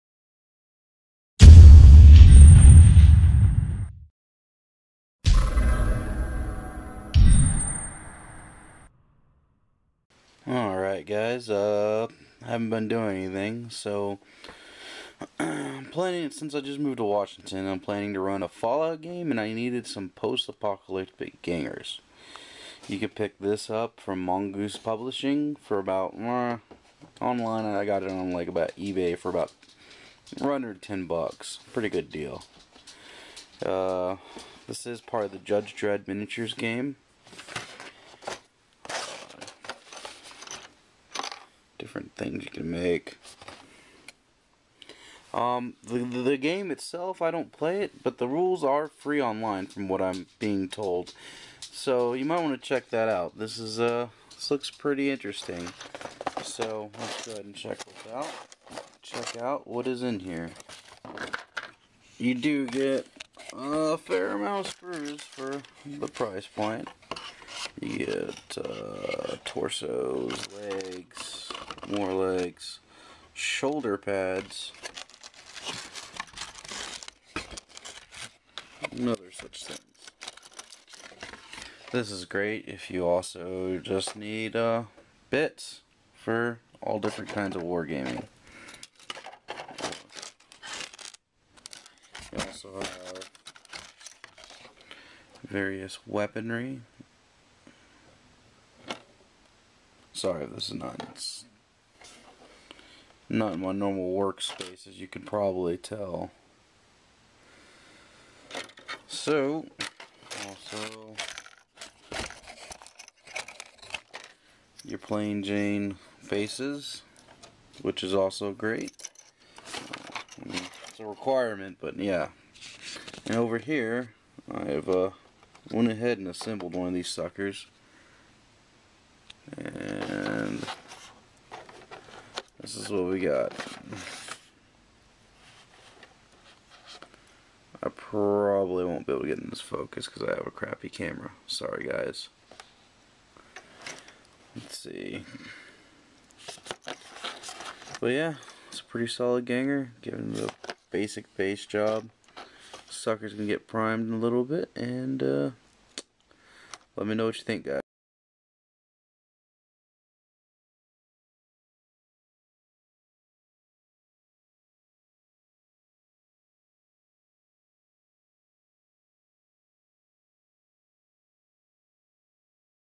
Unboxing: Gangs of Mega City One
BA Wayne unboxes some futuristic gangers from Mega City One for a game of FALLOUT!
Free Gangs of Mega City One Rules